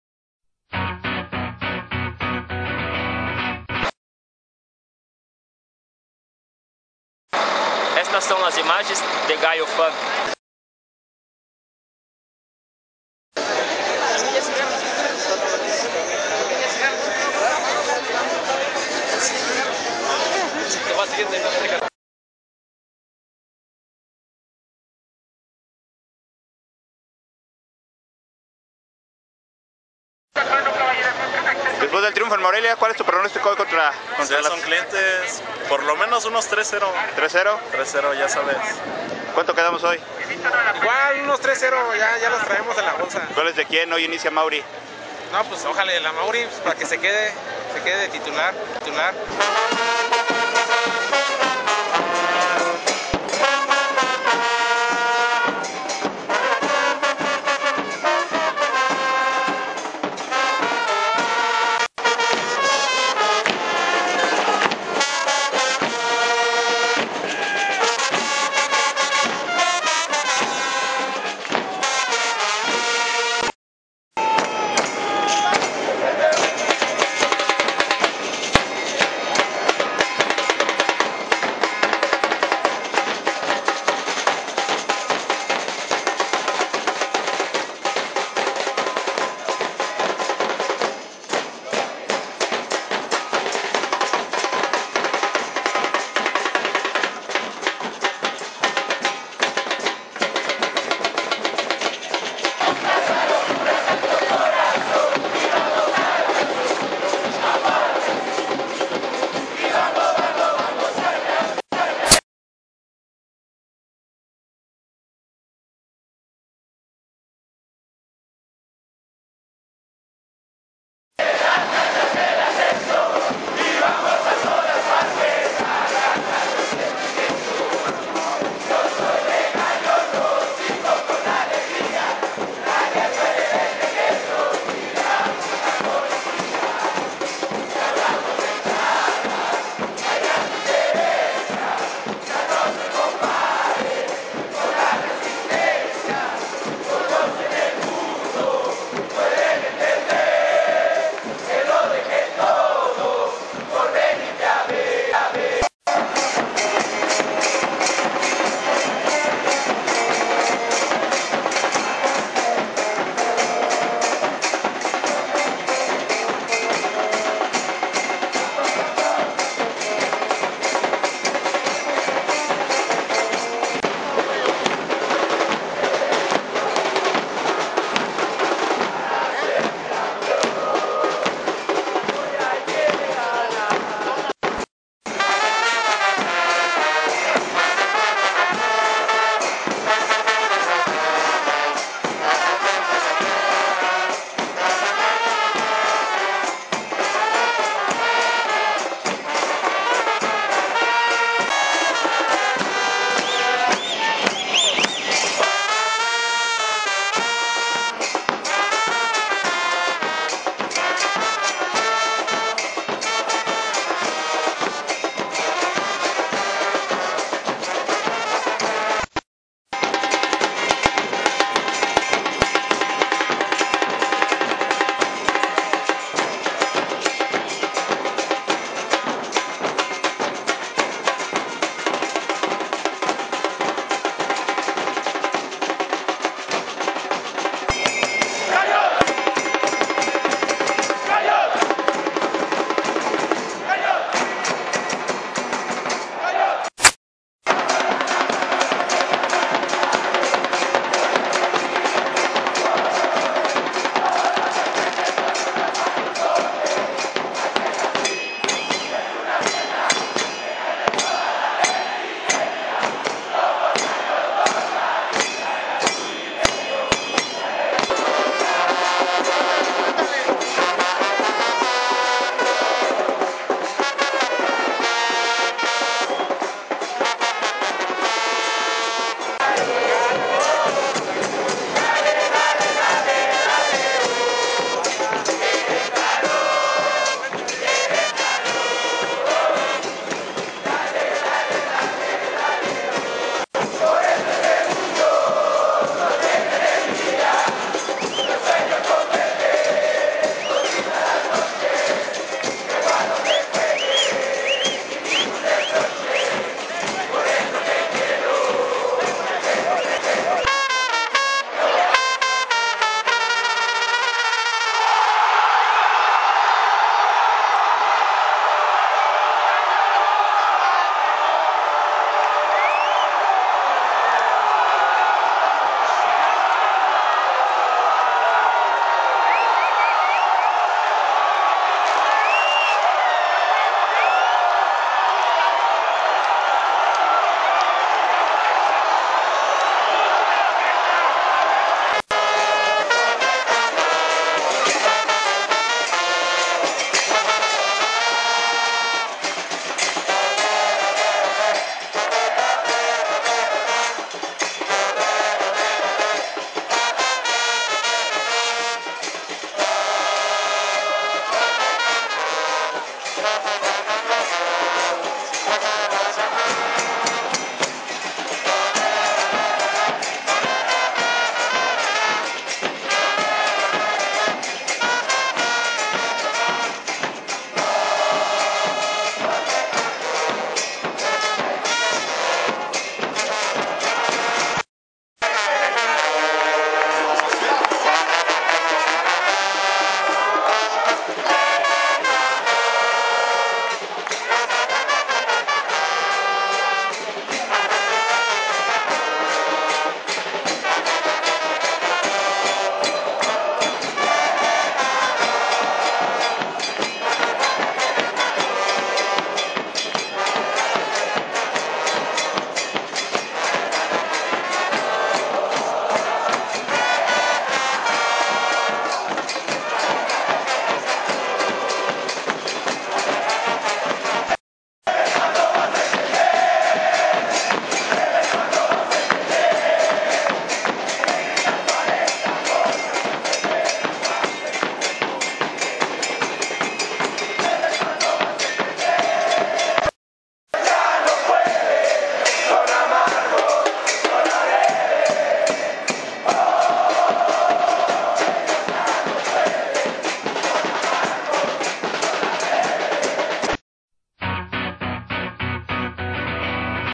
Gallofan / El triunfo de Gallos Blancos sobre Pumas desde la Tribuna Norte / Cl´14
Como se vivió el triunfo de Gallos Blancos sobre Pumas desde la Tribuna Norte del Estadio Corregidora.